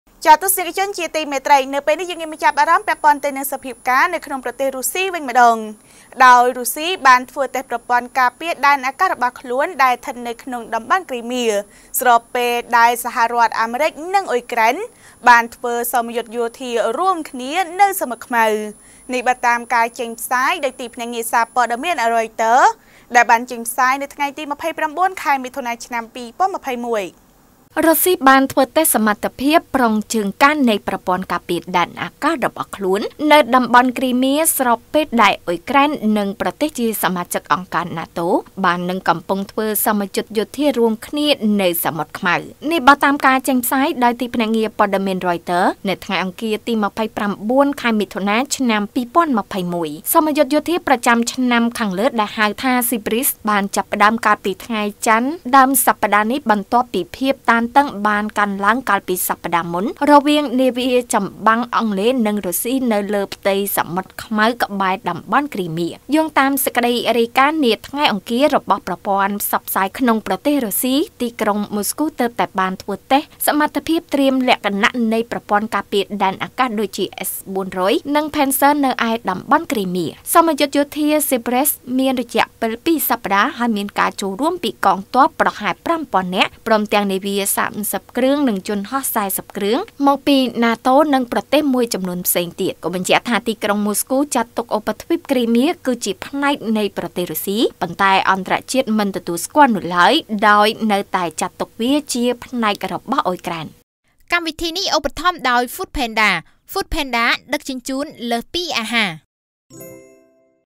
រុស្ស៉ីធ្វើតេស្តប្រព័ន្ធការពារដែនអាកាសនៅតំបន់គ្រីមៀ ចំពេលដែលអាមេរិកនិងអ៊ុយក្រែន ធ្វើសមយុទ្ធយោធា...
រុស្ស៉ីធ្វើតេស្តប្រព័ន្ធការពារដែនអាកាសនៅតំបន់គ្រីមៀ ចំពេលដែលអាមេរិកនិងអ៊ុយក្រែន ធ្វើសមយុទ្ធយោធានៅសមុទ្រខ្មៅ
* រាយការណ៍ព័ត៌មានដោយកញ្ញា ថាន  រស្មី  និងអ្នកនាង កេត សត្យា, ទូរទស្សន៍ព័ត៌មាន Fresh News